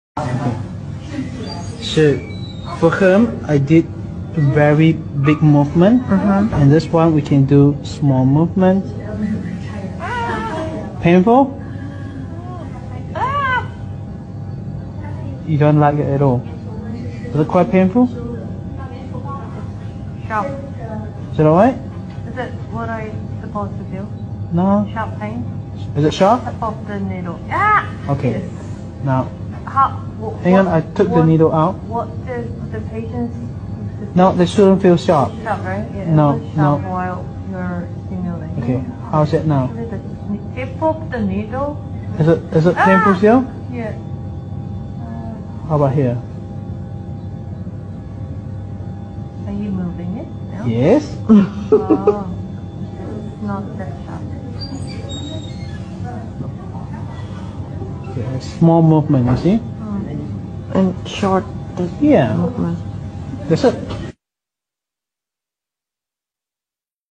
How to preform needle manipulation techniques on scalp? What should you do if patient feels sharp p
How to preform needle manipulation techniques on scalp?

What should you do if patient feels sharp pain while scalp acupuncture ?

The workshop of New Balance Method Acupuncture will host in the following countries

Pulse Balance Acupuncture workshop will be in London in April 2024.